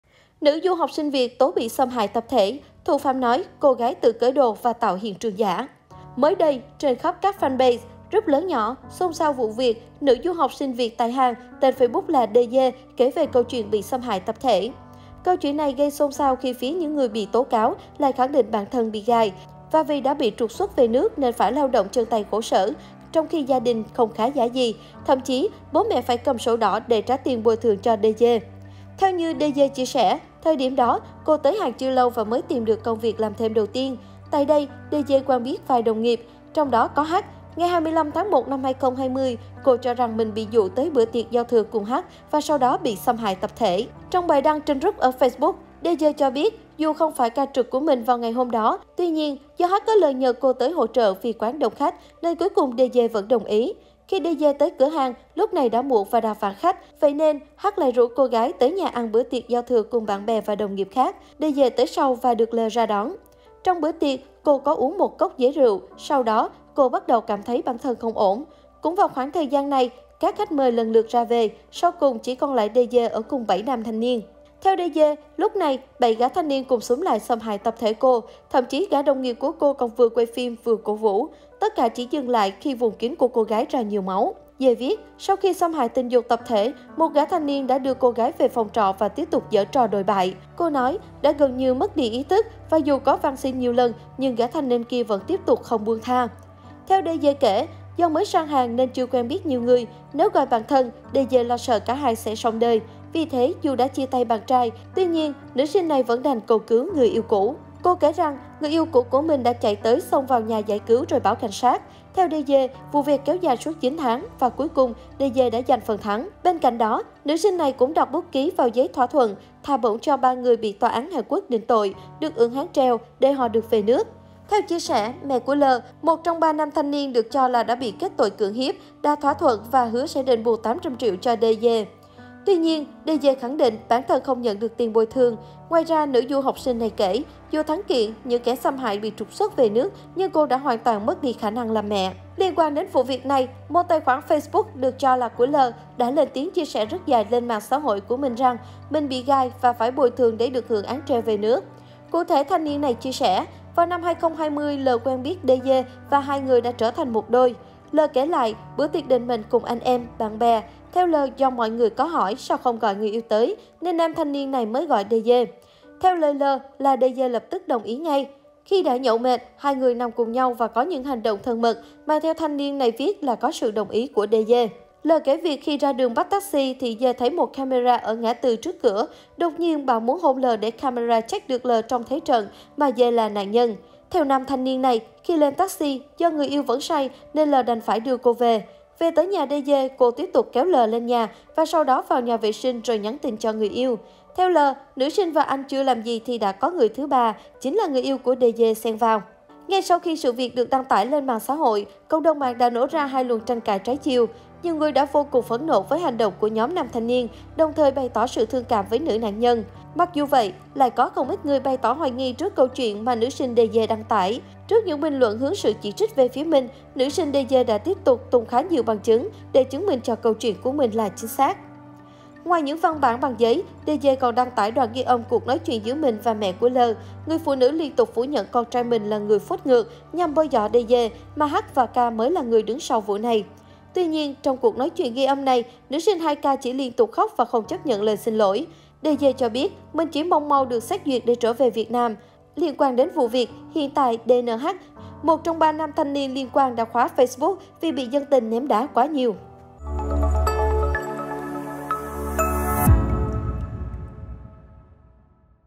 Nữ du học sinh Việt tố bị x.â.m h.ạ.i tập thể, thủ phạm nói cô gái tự cởi đồ và tạo hiện trường giả
Mới đây, trên khắp các fanpage, group lớn nhỏ xôn xao vụ việc nữ du học sinh Việt tại Hàn tên Facebook là Đ.D kể về câu chuyện bị x.â.m h.ạ.i tập thể.
Câu chuyện gây xôn xao khi phía những người bị tố cáo lại khẳng định bản thân bị "gài", và vì đã bị trục xuất về nước nên phải lao động chân tay khổ sở, trong khi gia đình không khá giả gì. Thậm chí, bố mẹ phải cầm cố sổ đỏ để trả tiền bồi thường cho Đ.D.